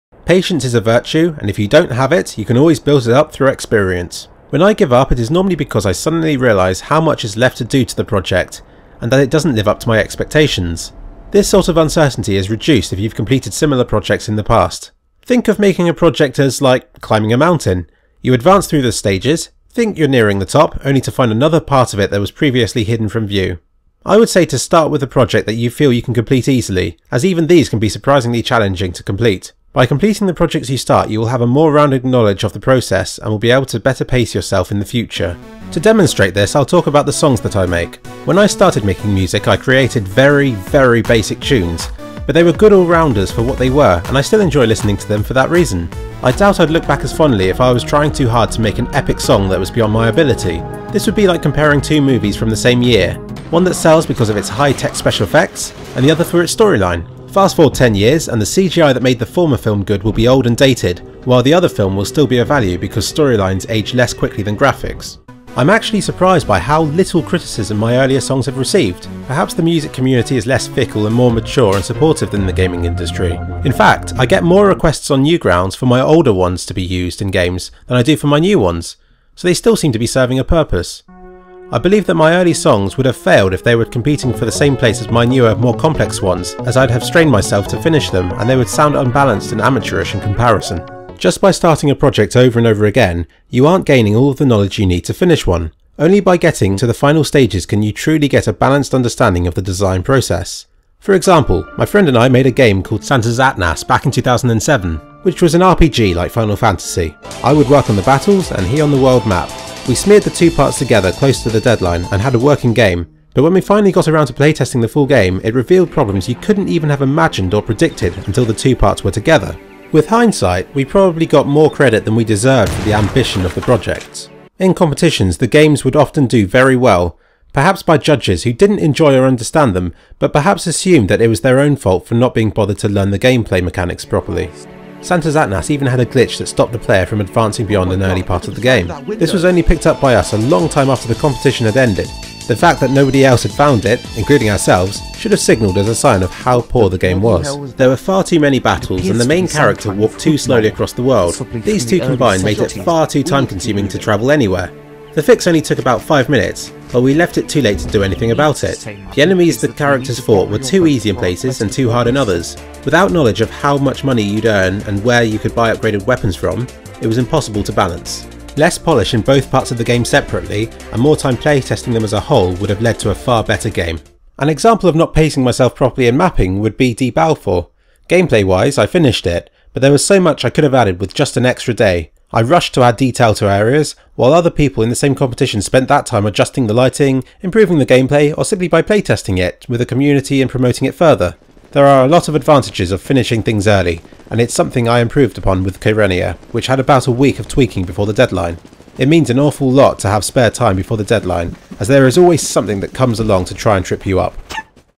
How to finish things - Patience by experience
Struggling to finish things? Go away and make some easier things until you level up your patience skill! This video is mainly for the audio, the graphics are simply projects of mine that seem to fit the theme.